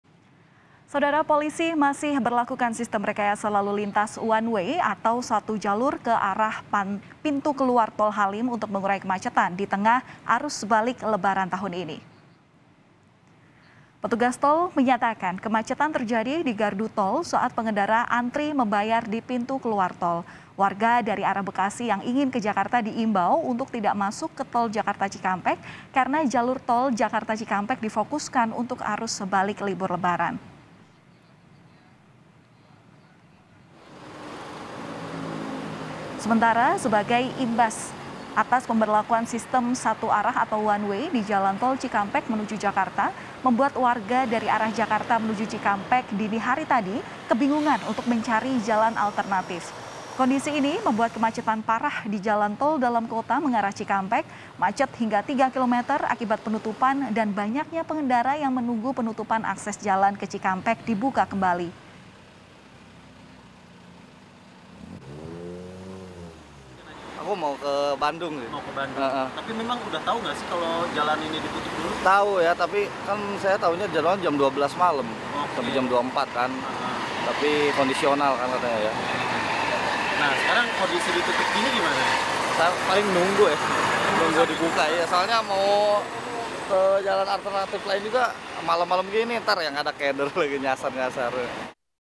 Warga Mengaku Kebingungan Cari Jalan Alternatif Imbas Sistem One Way di Tol Cikampek-Jakarta
JAKARTA, KOMPAS.TV - Polisi masih berlakukan sistem rekayasa lalu lintas "One Way" atau satu jalur ke arah Pintu Keluar Tol Halim untuk mengurai kemacetan di tengah arus balik lebaran tahun ini.

Petugas tol menyatakan, kemacetan terjadi di gardu tol saat pengendara antre membayar di pintu keluar tol.

Warga dari arah Bekasi yang ingin ke Jakarta diimbau untuk tidak masuk ke Tol Jakarta-Cikampek, karena jalur Tol Jakarta-Cikampek difokuskan untuk arus balik lebaran.

Sementara, imbas pemberlakuan sistem satu arah di Jalan Tol Cikampek menuju Jakarta, membuat warga dari arah Jakarta menuju Cikampek, Sabtu dini hari tadi (7/5), kebingungan mencari jalan alternatif.

Kondisi ini, membuat kemacetan parah di Jalan Tol Dalam Kota mengarah Cikampek macet hingga 3 kilometer akibat penutupan dan banyaknya pengendara yang menunggu penutupan akses jalan ke Cikampek dibuka kembali.